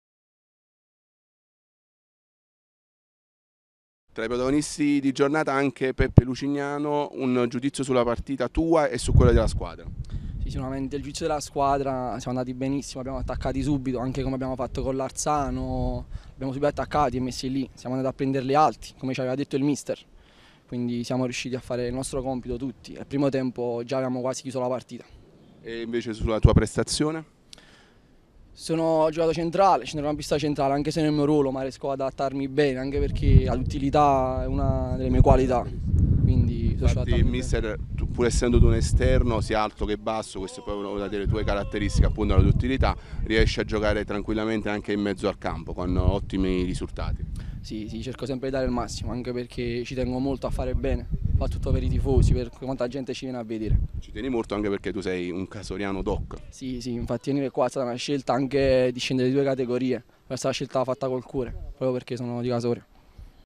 Casoria Cellole 4 - 0 Intervista Peppe Lucignano
Campionato di Prima Categoria Campania 2015/2016
21^ giornata - Girone A

Le dichiarazioni post partita di Giuseppe Lucignano
Ufficio Stampa
Casoria Calcio